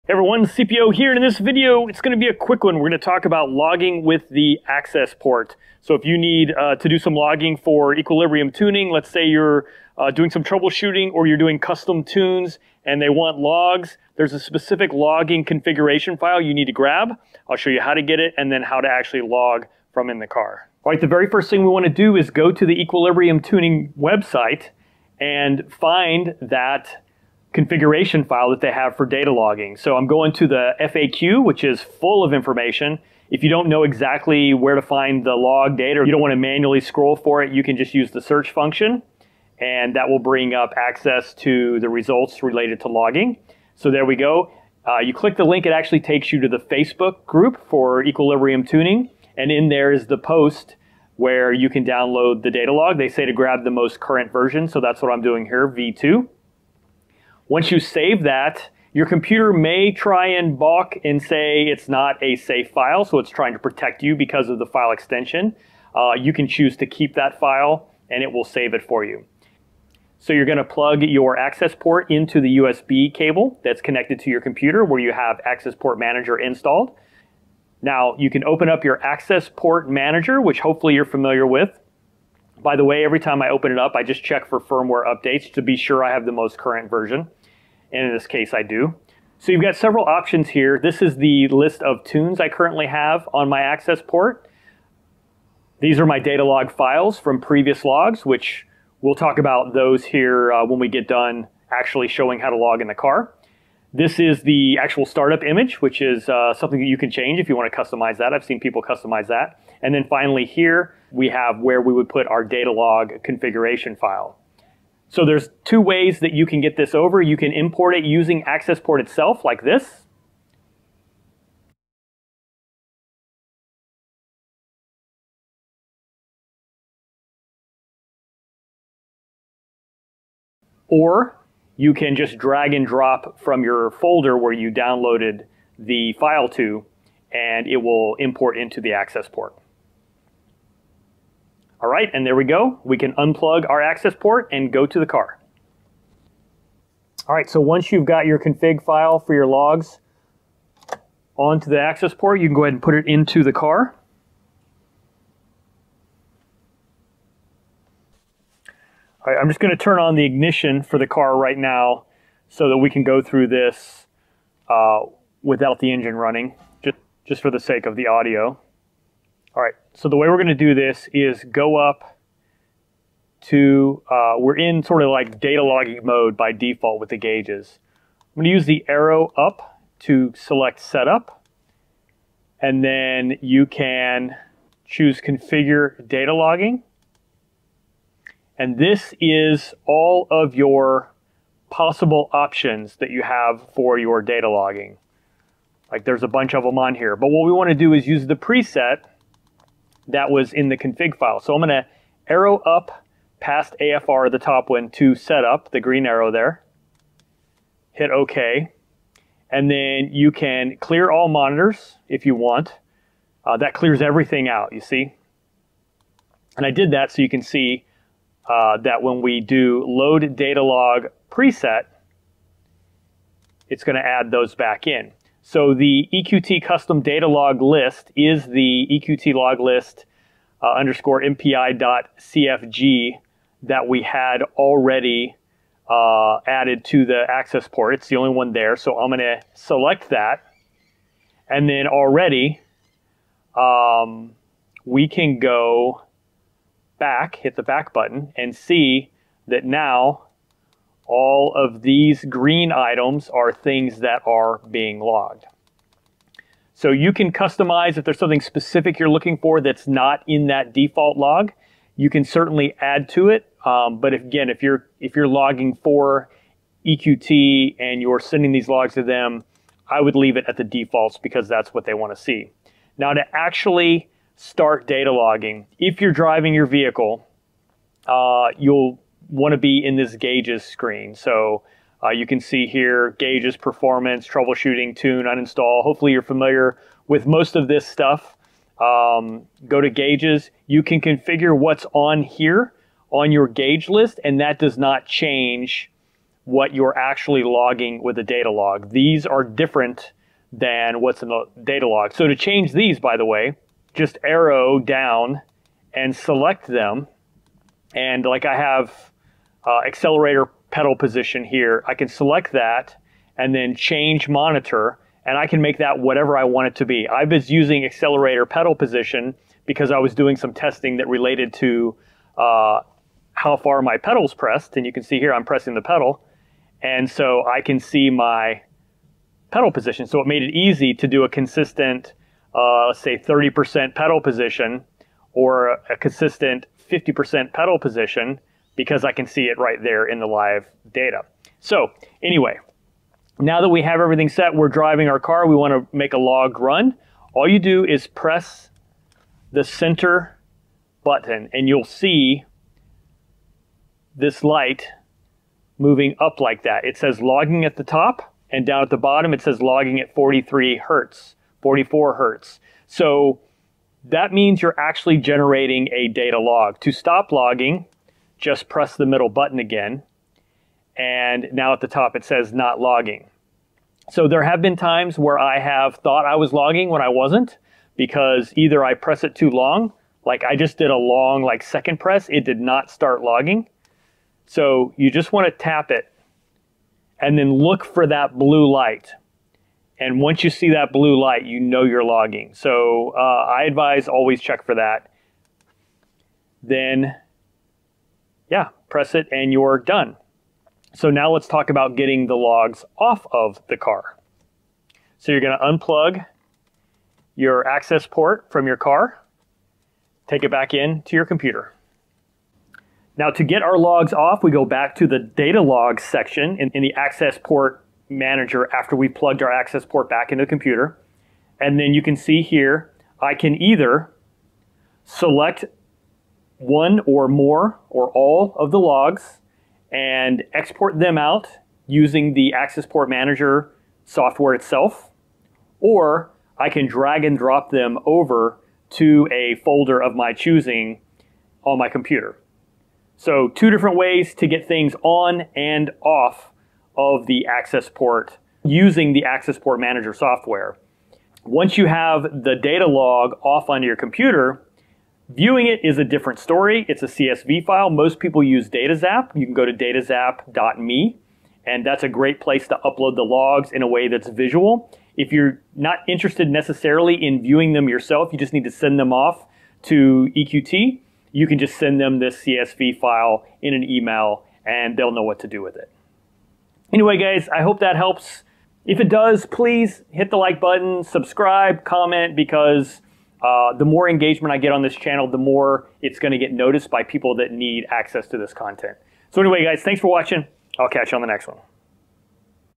How do datalog with COBB Accessport with EQT Custom Logging Configuration
If you are grabbing logs for EQT (Equilibrium Tuning) you are going to want to use their custom logging configuration so that you can be sure you are getting the data they need to see. This video shows how, but is also helpful for COBB Accessport data logging in general.